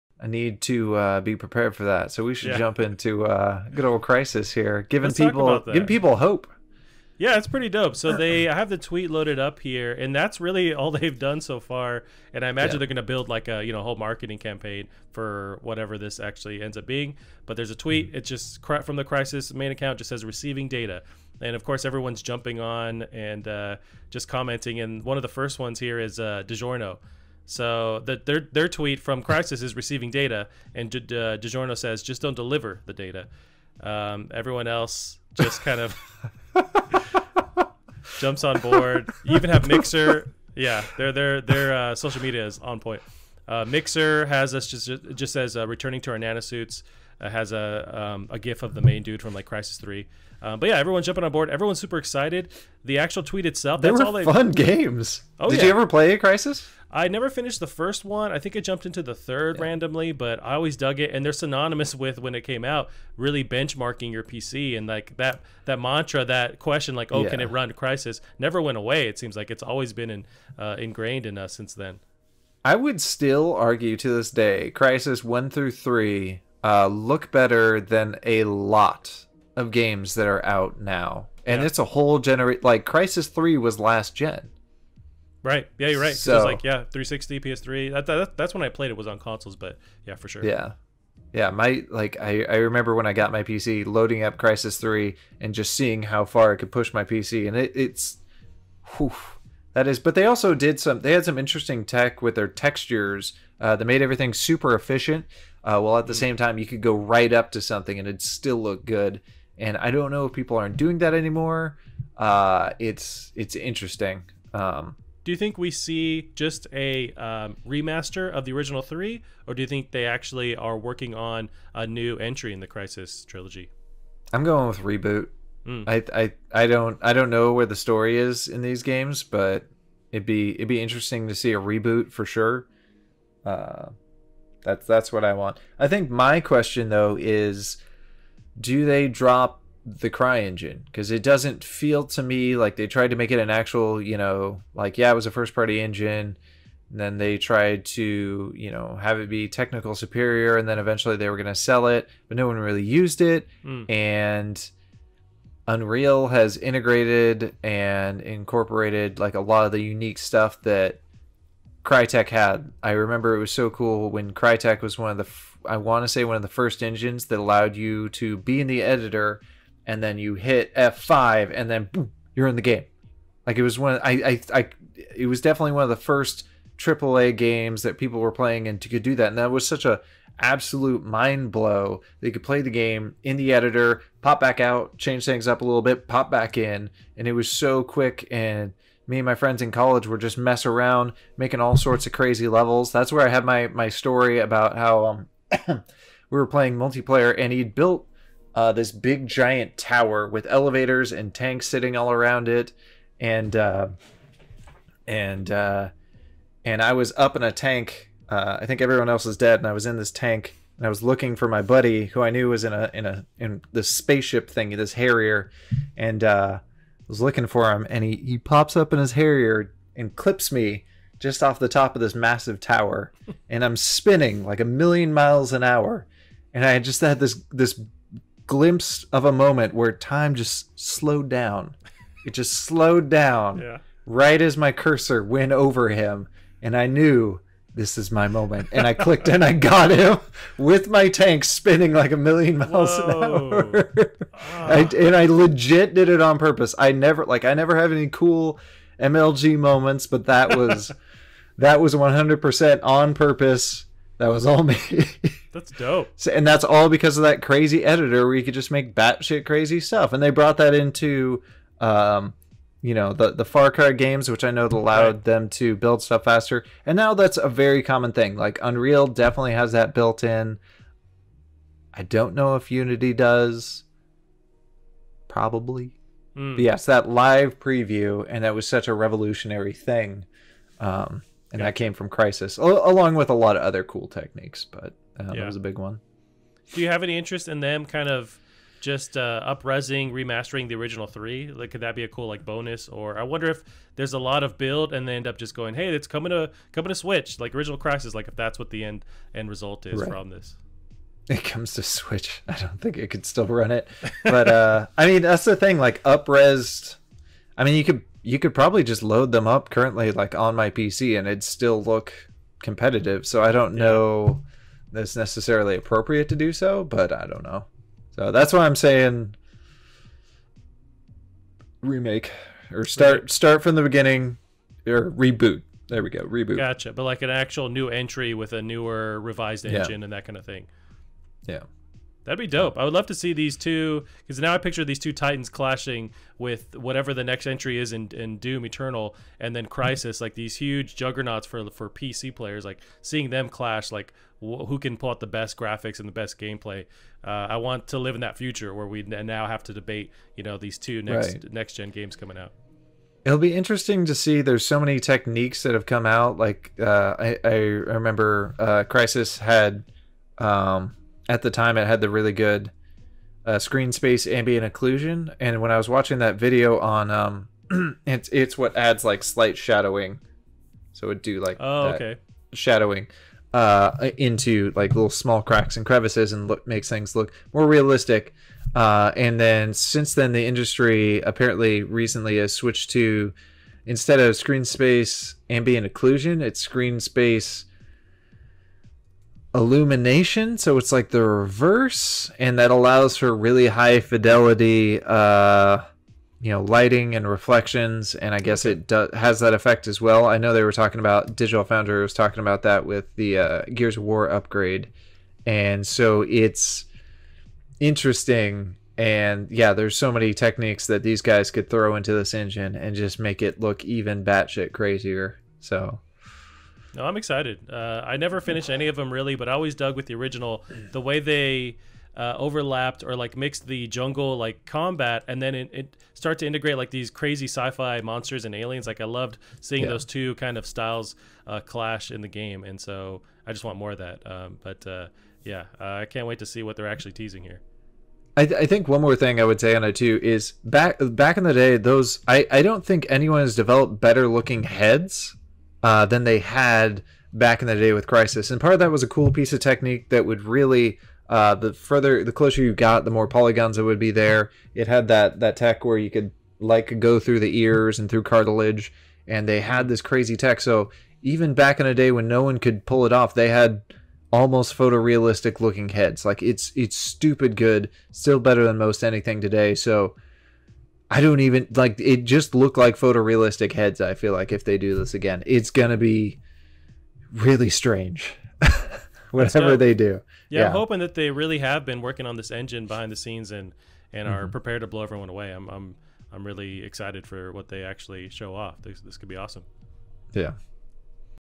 Crysis Is Coming Back! | Plastic Hearts #61
The Nano-Suit Boi's are back to look back on Crysis and breakdown the recent tweets from the official Crysis account.  Lets get into it!

We greatly appreciate it.